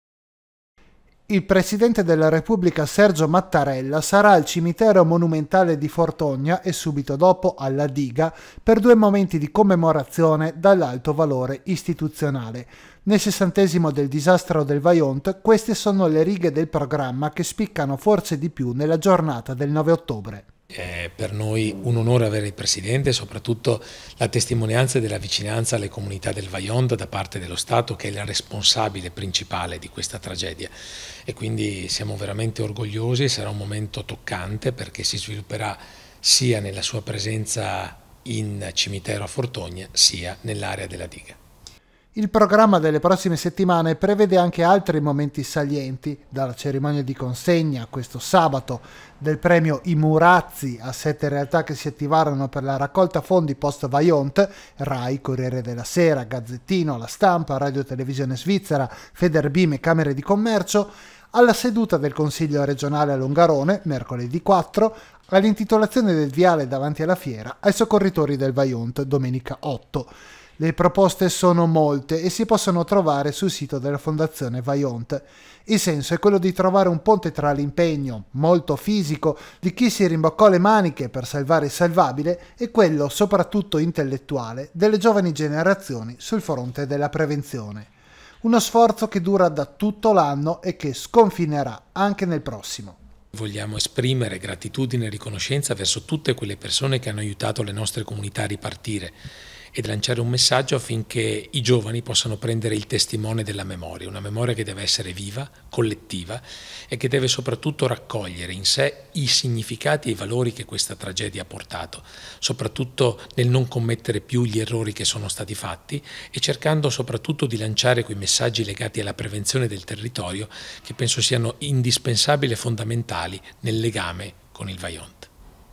Sessantesimo del Vajont: il presidente della Repubblica Sergio Mattarella torna a Longarone
Sarà presente a due momenti dall'alto valore simbolico e istituzionale